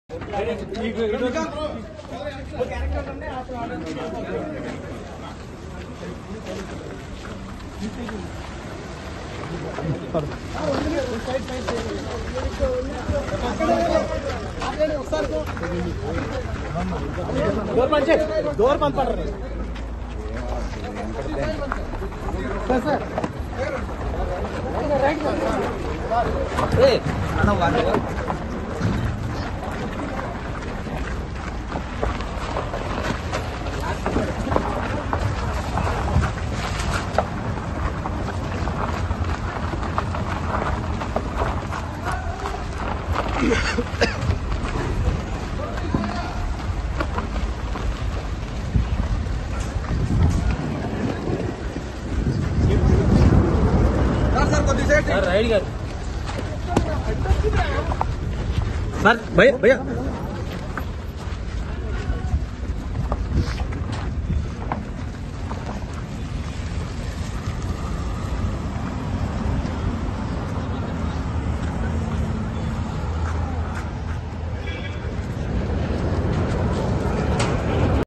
Ambati Rayudu Came Outside From Janasena Party Office After Meeting With Pawan Kalyan | TC Brother
Ambati Rayudu Came Outside From Janasena Party Office After Meeting With Pawan Kalyan